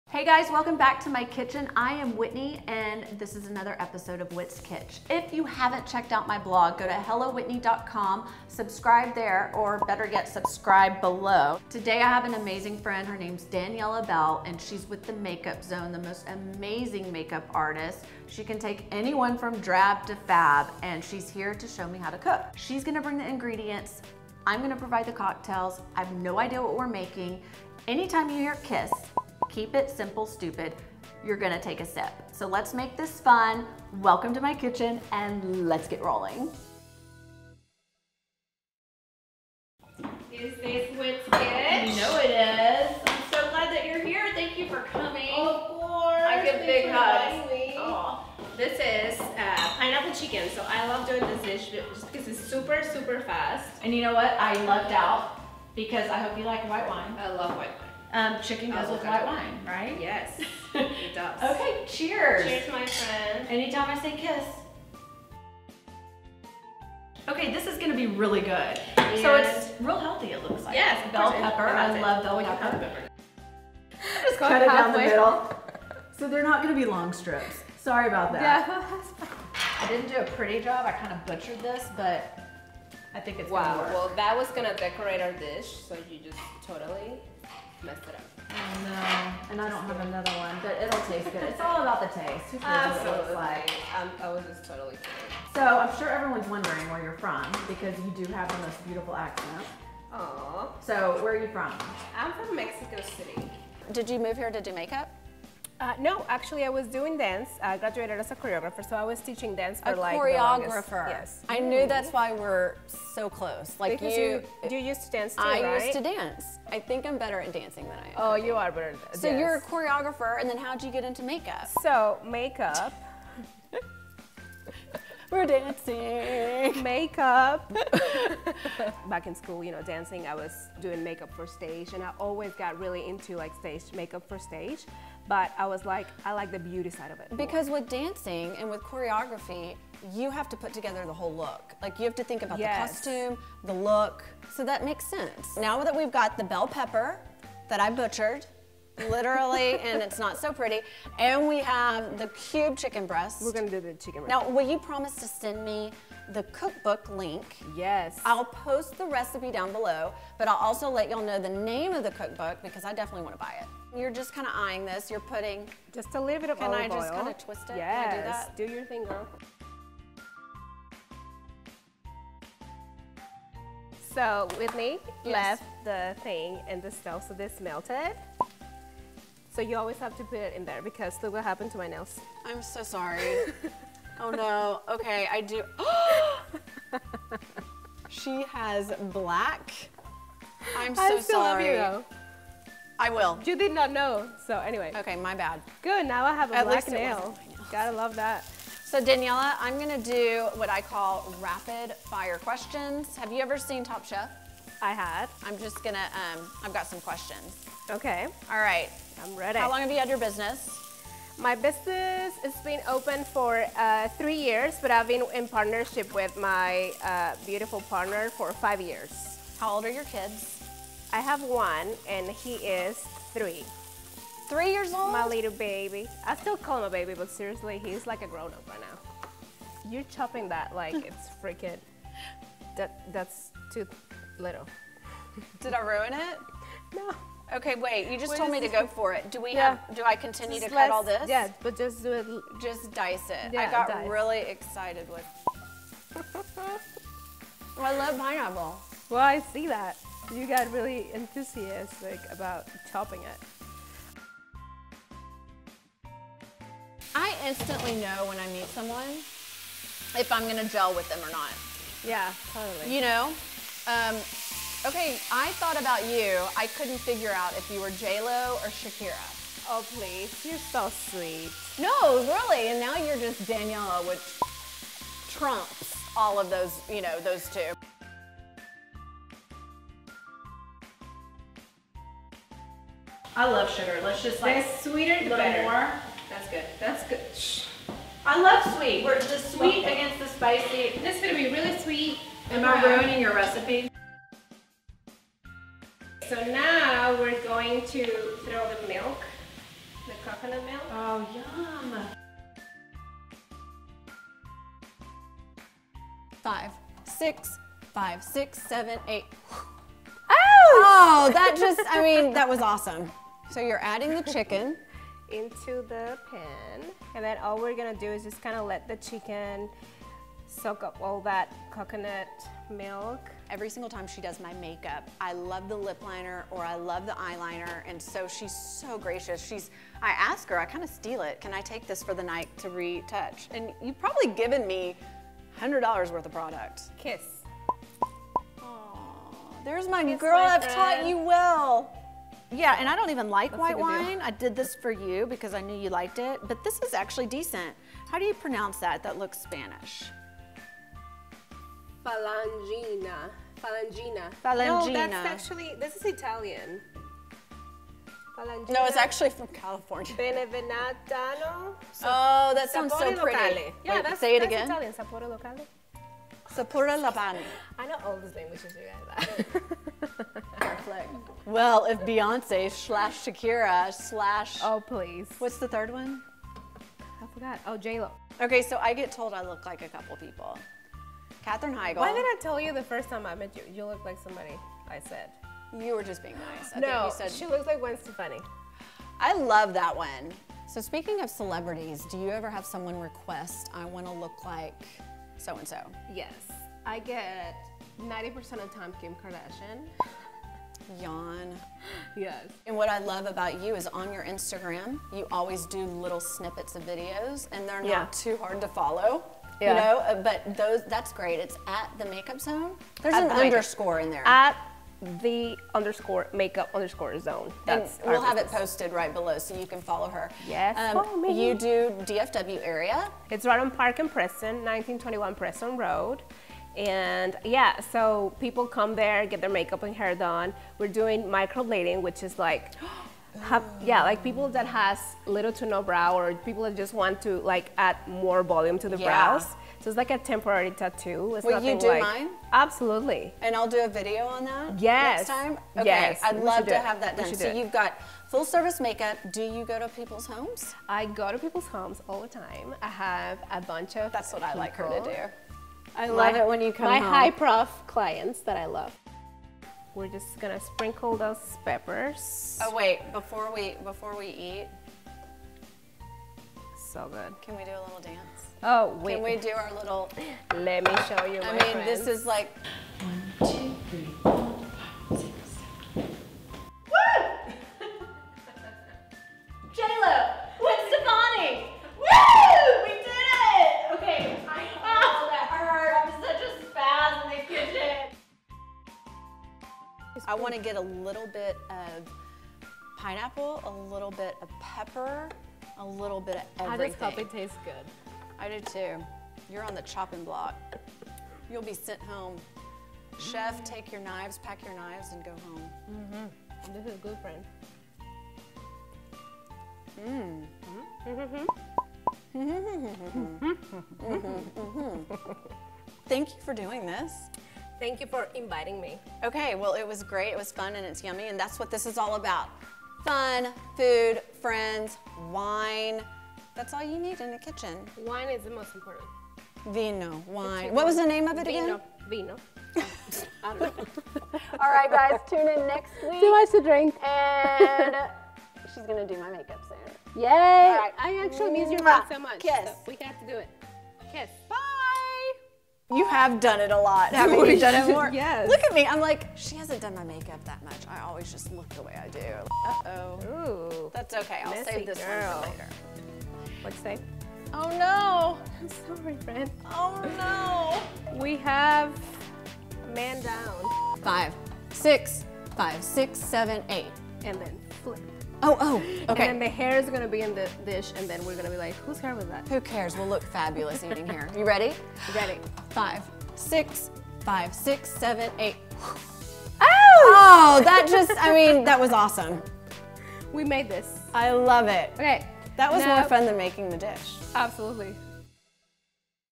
WHIT's KITCH | Episode 4 | Pineapple Chicken with Daniela Bell
Cooking, chatting and laughing with my friend - DANIELA BELL - owner of The Makeup Zone here in Dallas, TX. She's worked with stars on Real Housewives stars (RHOD and RHONW) and many other celebrities.  And today we're making easy pineapple chicken!

         insta @HelloWhitneyXO | FB @HelloWhitneyK
            twitter @HelloWhitneyK | Pinterest @HelloWhitneyK

RECIPE: Healthy Pineapple Chicken
1.5 lbs chicken breast
1/2 tsp salt
1/2 tsp curry seasoning
2 tbl EVO
1 cup diced pineapple
1 red pepper cut into bite sized strips
3/4 cup unsweetened coconut milk
1 tbl brown sugar

Sprinkle chicken with salt and curry seasoning
In large skillet, 1 tbl EVO on high heat
Add chicken and saute and toss until browned evenly
In second skillet, 1 tbl EVO on high heat
add bell peppers and pineapple - saute for 3 minutes
mix in coconut milk and brown sugar for 1 minute
add mixture to chicken skillet and let simmer medium heat for 2 min